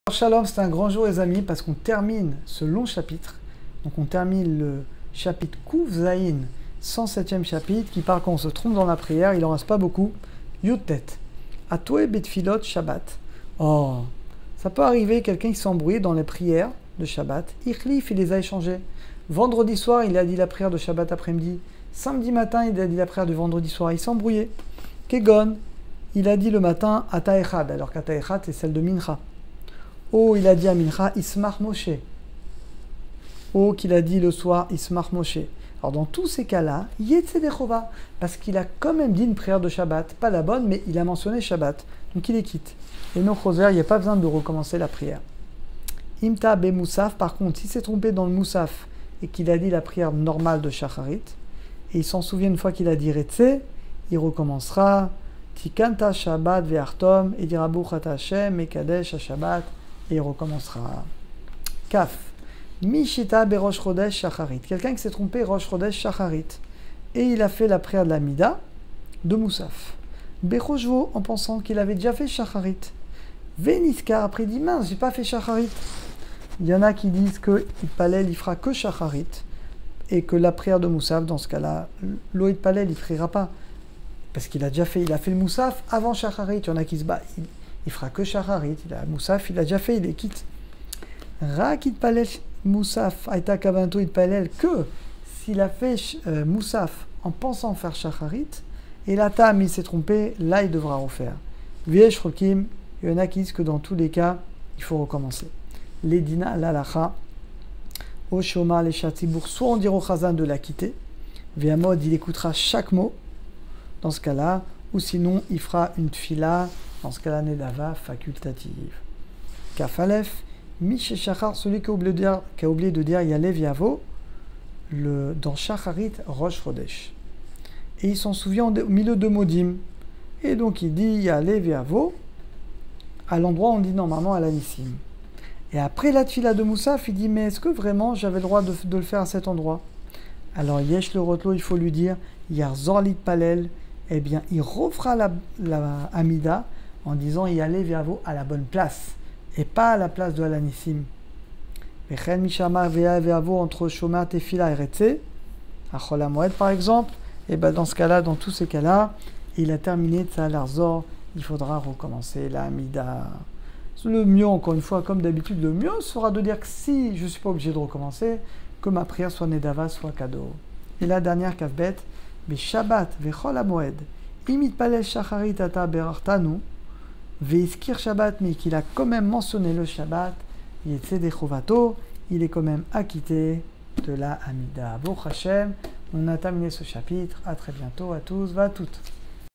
YALKOUT YOSSEF ONLINE - SIMAN 107c - Celui qui se trompe dans sa prière / HALAKHOT 19 ... 22
Rav Bensimon a le plaisir de vous présenter son nouveau projet " YalKout Yossef OnLine"